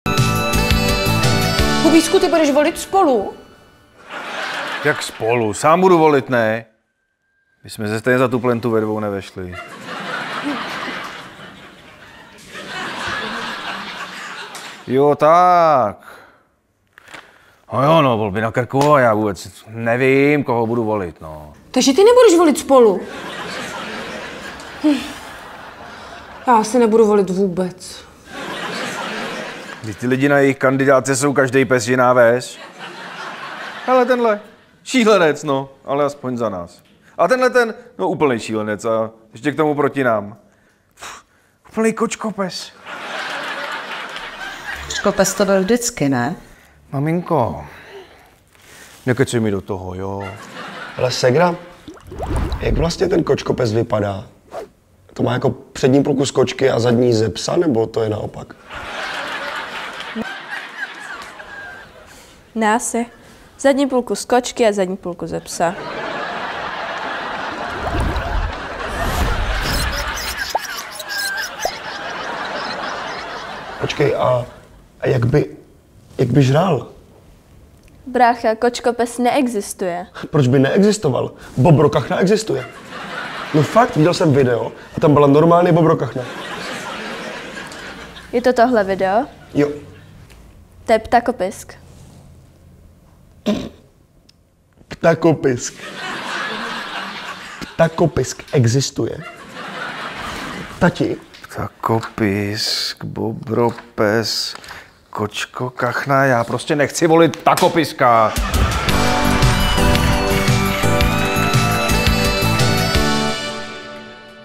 Korzárovi - 1. díl
Doufali jste, že to neuděláme, co? Je tu první díl slibovaného sitcomu. A myslíme, že překoná i takové pecky, jako byl Premiér Jaromíra Soukupa a loňský šlágr Eliška a Damián. 😍

Tak co, kolik bobrokachen nám zvýší dosah?